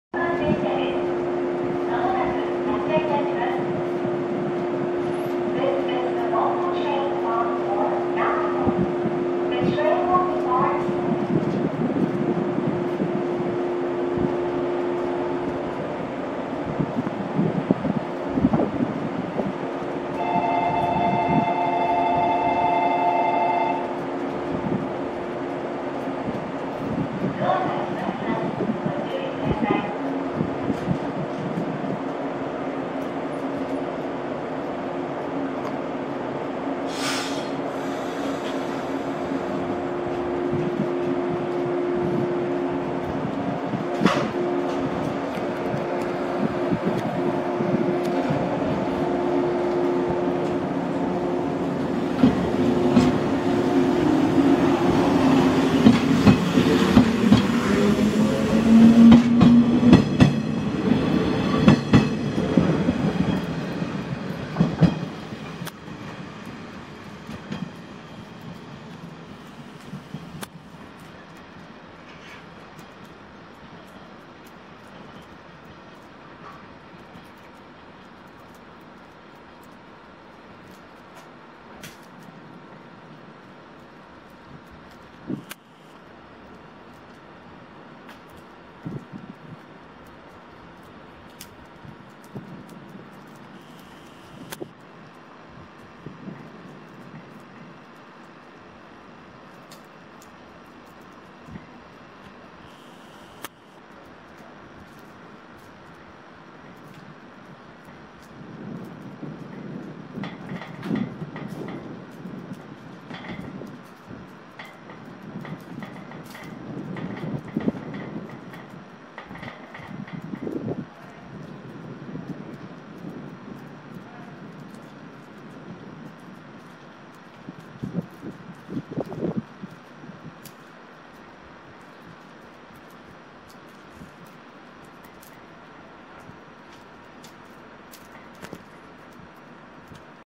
【発車シーン】雪の妙高高原駅を発車するしなの鉄道115系
今や貴重な115系が妙高高原を出発し、雪の中長野へ向けて進みます。
雪景色に115系は似合うと思います。